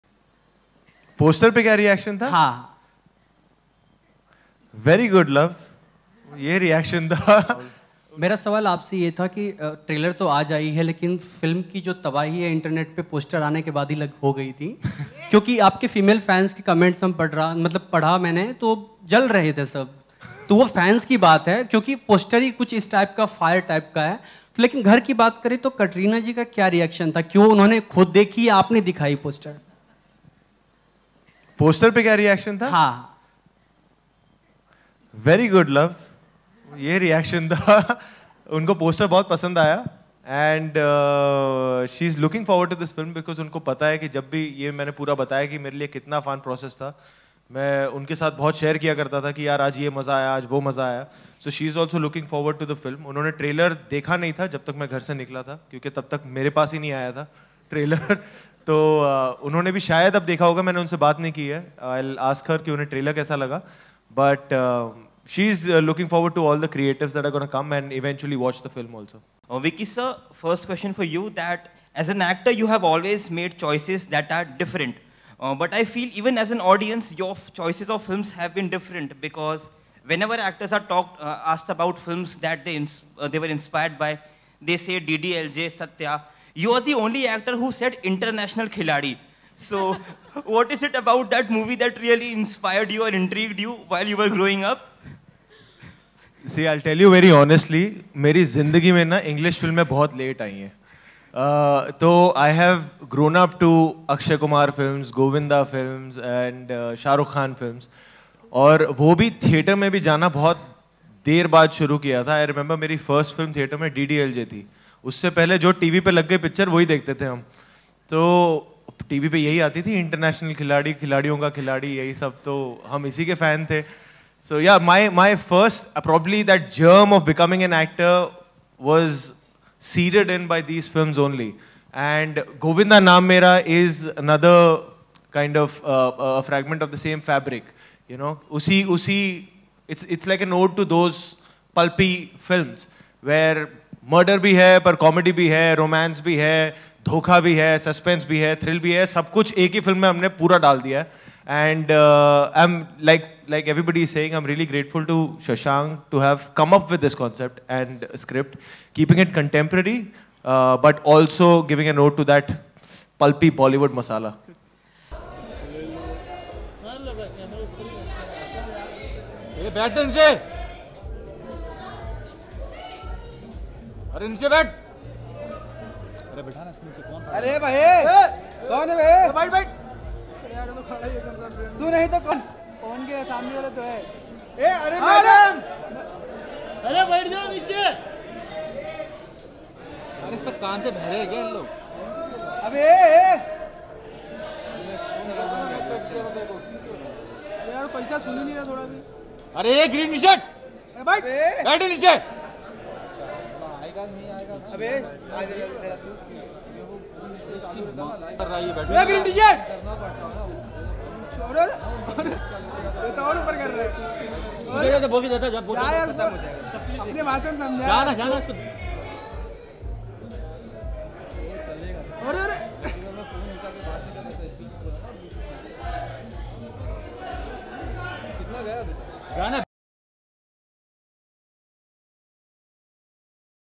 Vicky Kaushal Reveals Katrina Kaif's Reaction To The 'Govinda Naam Mera' Poster
Vicky Kaushal, Bhumi Pednekar and Kiara Advani are all set to win audience’s heart with their upcoming thriller-comdey ‘Govinda Naam Mera’. The makers dropped the trailer in an event with the lead cast and also Karan Johar hosting the same. On such occasion, Vicky was asked about his wife KatrinaKaif’s reaction to the movie poster. Watch the video to know about how the actress-wife reacted as it turns out to be a laugh riot.